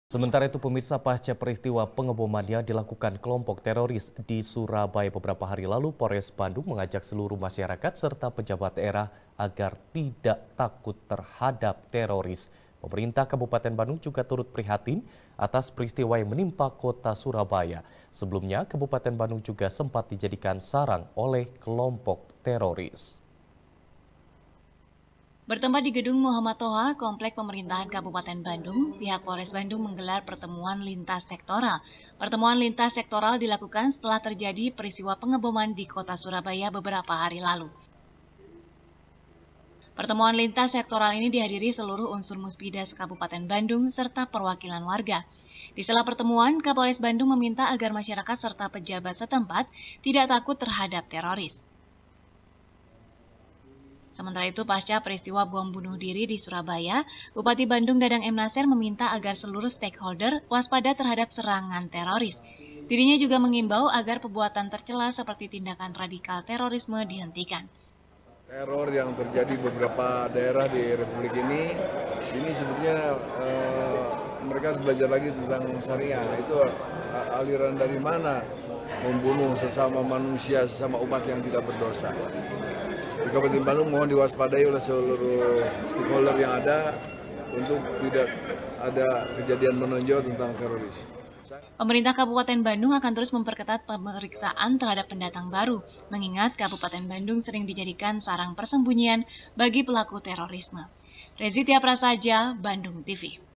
Siaga Teroris, Bupati Desak Perketat Pengamanan | SBR | BANDUNG TV
SBR | BANDUNG TV | IRFAN BUDIAWAN |Pasca peristiwa pengeboman yang dilakukan kelompok teroris di surabaya beberapa hari lalu, polres bandung mengajak seluruh masyarakat serta pejabat daerah, agar tidak takut terhadap teroris. pemerintah kabupaten bandung juga turut perihatin atas peristiwa yang menmpa kota surabaya. sebelumnya, kabupaten bandung juga sempat dijadikan sarang oleh kelompok teroris.